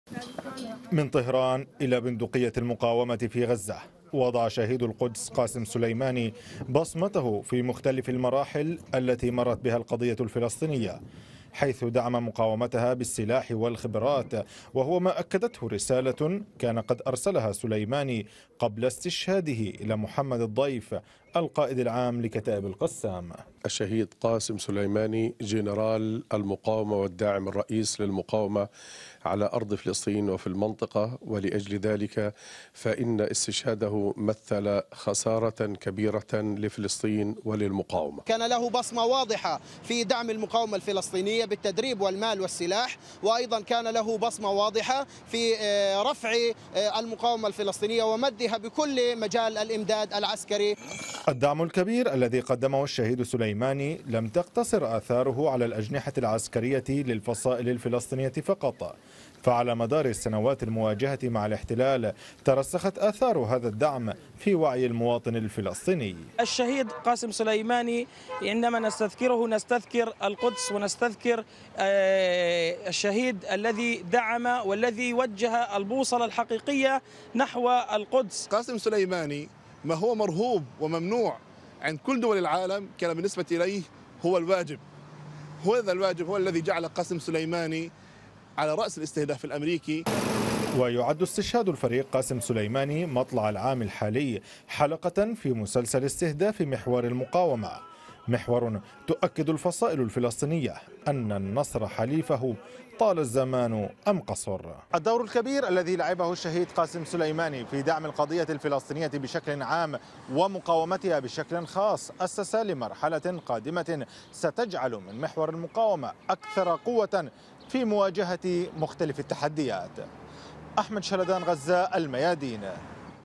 الشهيد قاسم سليماني دعم القضية الفلسطينية ومقاومتها بالسلاح والخبرات
إعداد: أحمد شلدان
الشهيد قاسم سليماني كان داعماً أساسياً للمقاومة في قطاع غزة، ومنذ استشهاده تؤكد المقاومة الفلسطينية أنها أكثر تماسكاً في مواجهة تحديات المرحلة.